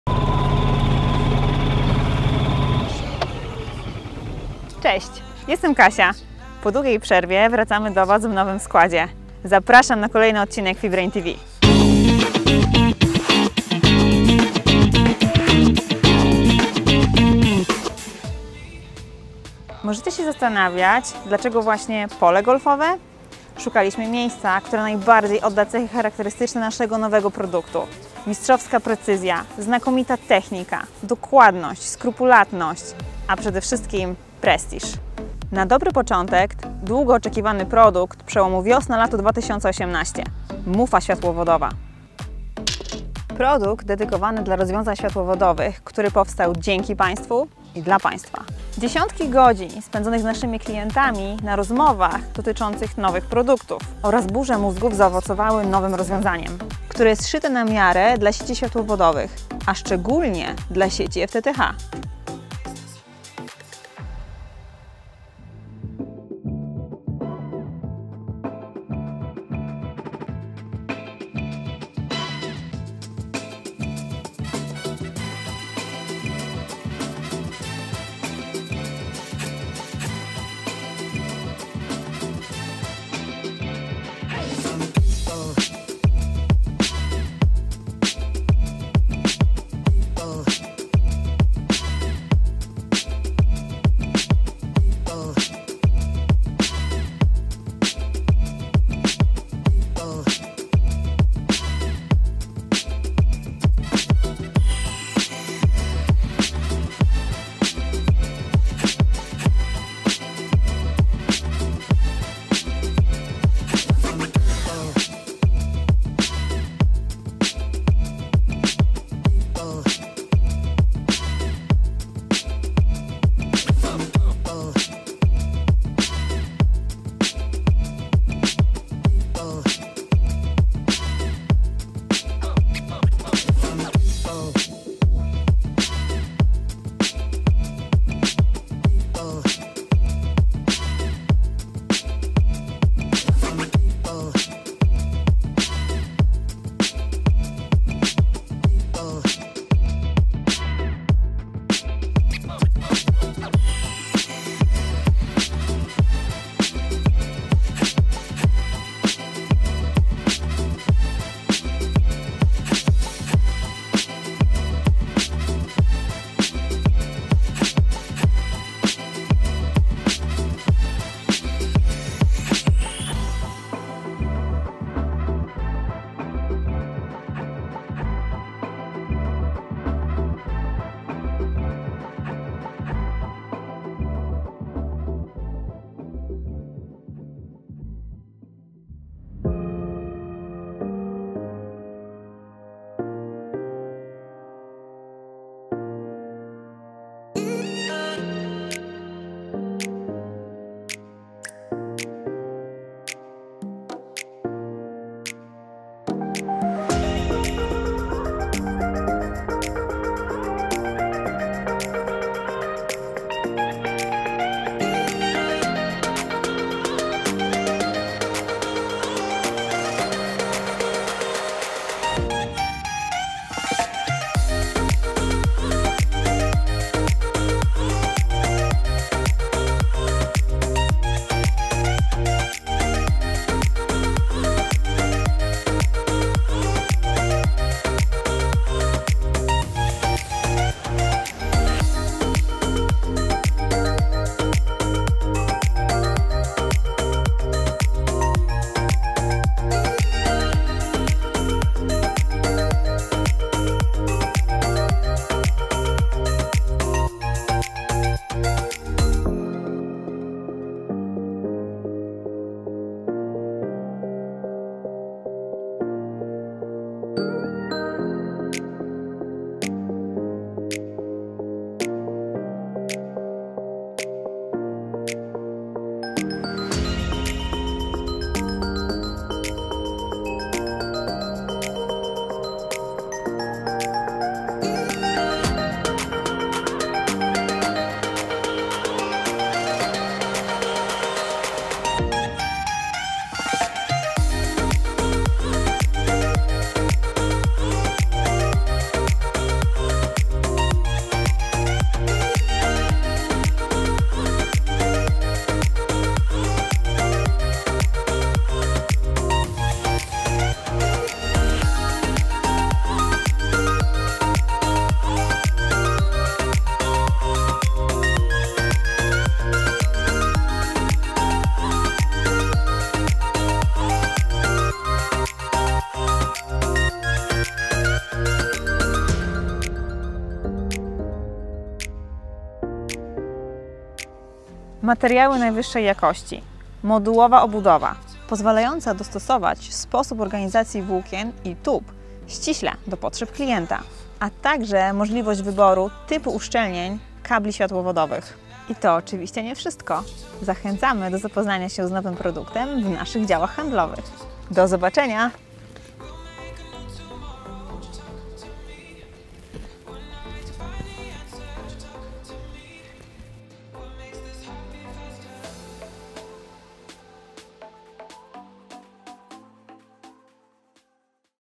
FIBRAIN TV - ODCINEK #6 - ŚWIATŁOWODOWA MUFA KAPTUROWA
Dziś w roli głównej występuje najnowsza, światłowodowa mufa kapturowa!
Dlaczego jest aż tak innowacyjna?
- Różne warianty wykonania
- Modularność
- Skalowalność
- Wiele zastosowań w różnych środowiskach pracy
- Łatwa i szybka instalacja

Dajcie łapki w górę – kto już czeka na kolejny odcinek FIBRAIN TV!